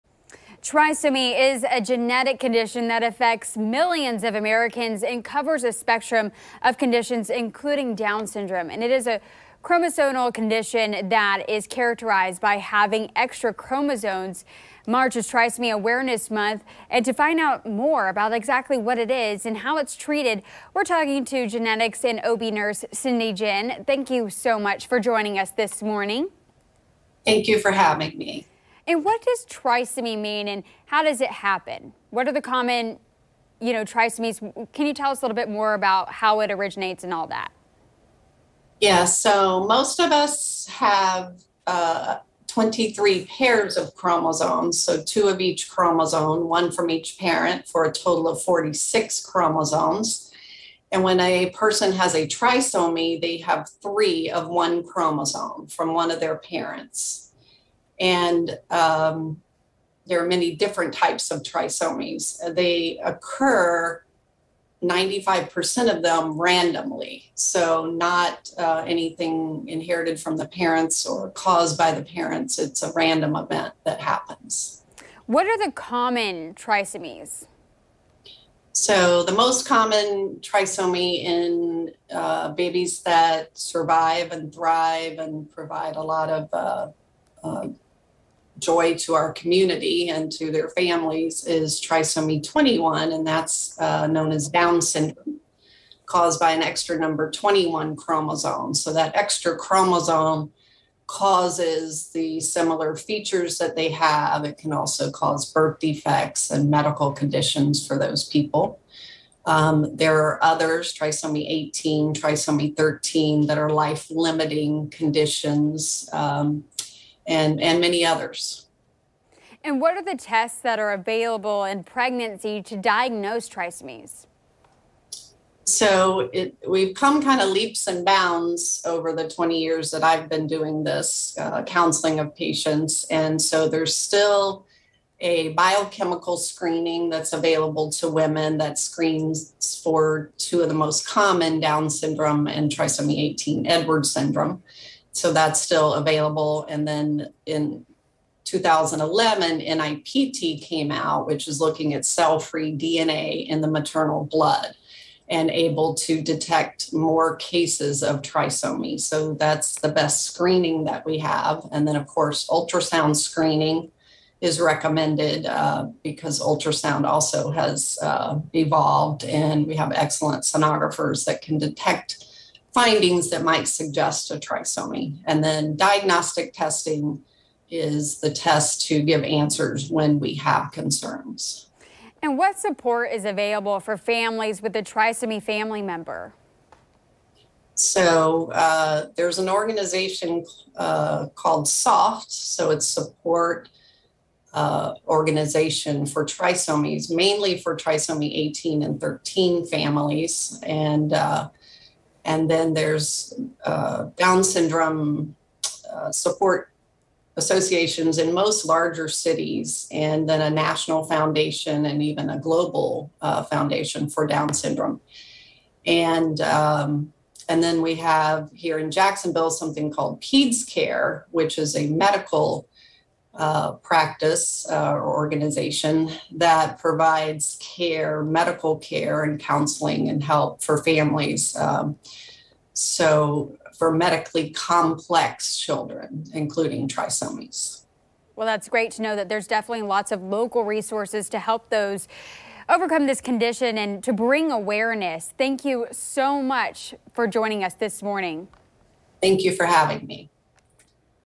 Raising awareness about trisomy
Genetics and OB nurse Cindy Ginn joins us to talk about the genetic condition known as trisomy, what causes it and how it can be treated.